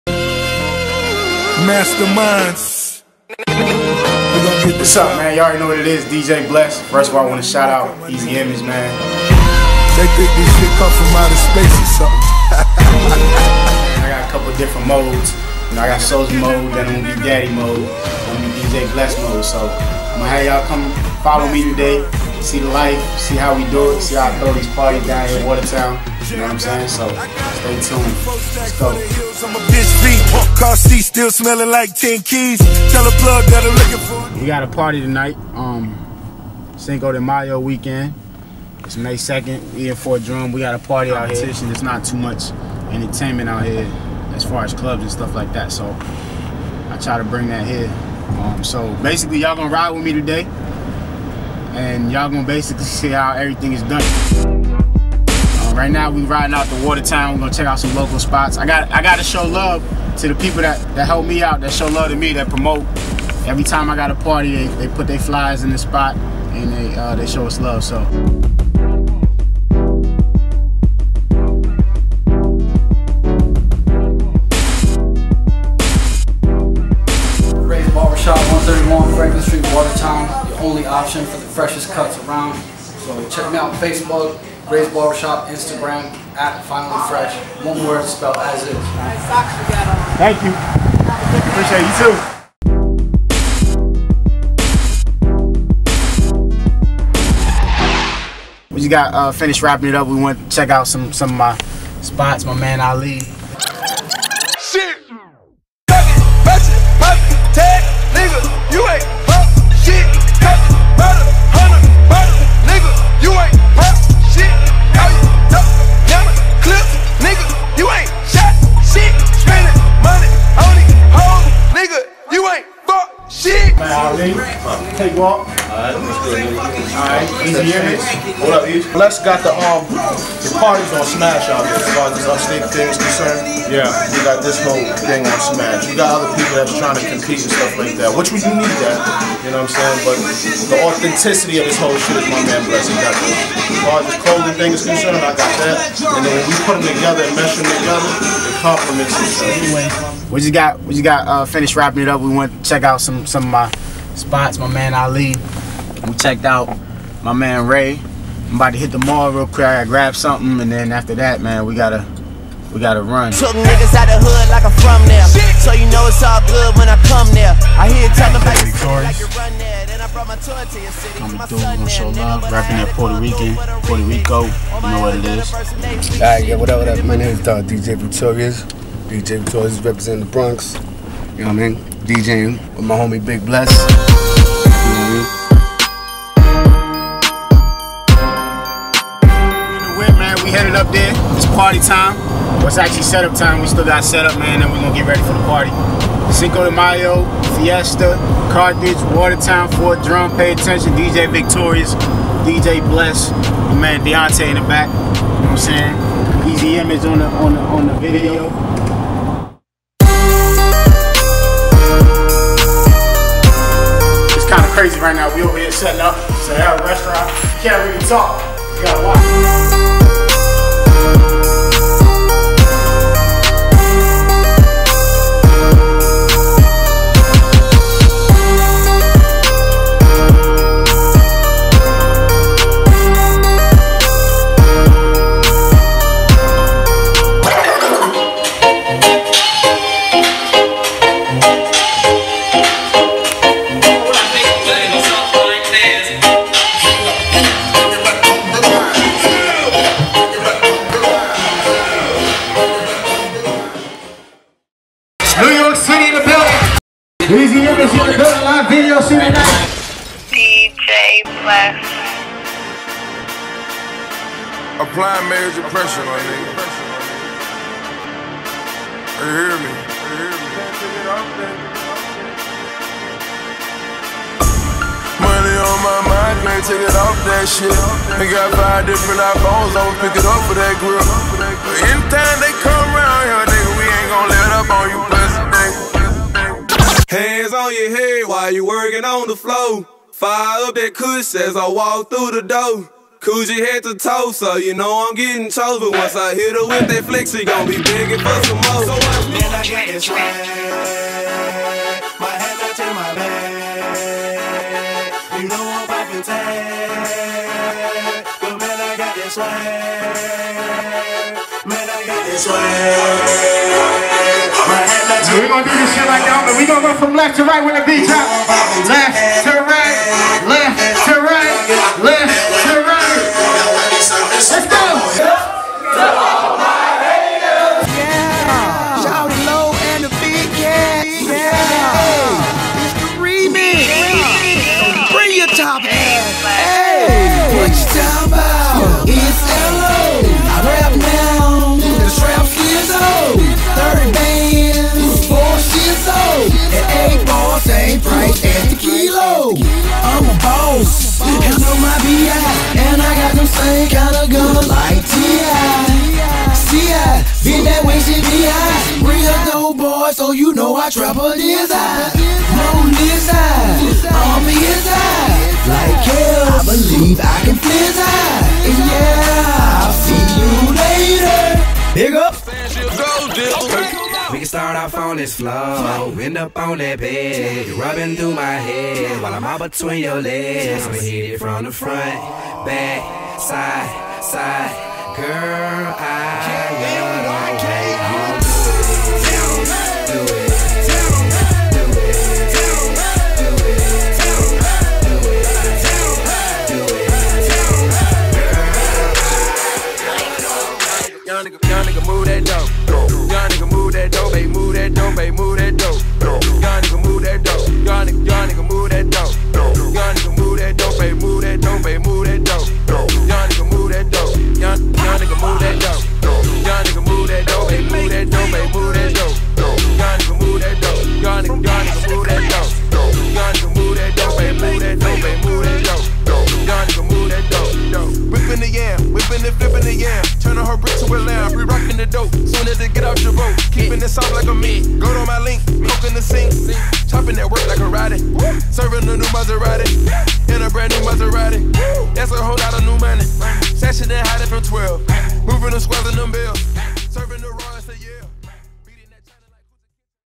DJ Bless- Cinco De Mayo (Hellafocus)
Follow DJ Bless throughout his day as he sets up yet another dope party